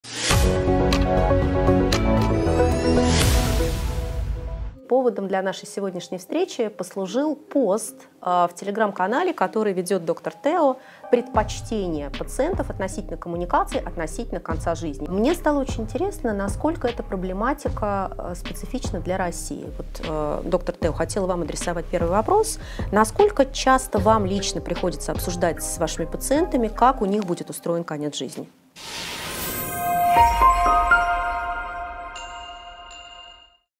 Терапия конца жизни. Трудности коммуникации | Премьера 30 июня
Часто ли мы обсуждаем с нашими пациентами терапию конца жизни?

По нашему внутреннему представлению обсуждение терапии конца жизни стоит особняком, это почти что табуированная тема.

С одной стороны, нам понятен страх врачебного сообщества перед неизлечимыми и прогрессирующими заболеваниями, с другой – у пациентов может быть иное восприятие смерти и всего что с этим может быть связано.

Анна Хасина вместе с Ойбеком Тургунхужаевым и Ганной Маменко обсудят исследования, где была попытка проанализовать пациентскую перспективу о таких вопросах как терапия конца жизни, завещание мозга для исследований и т.д.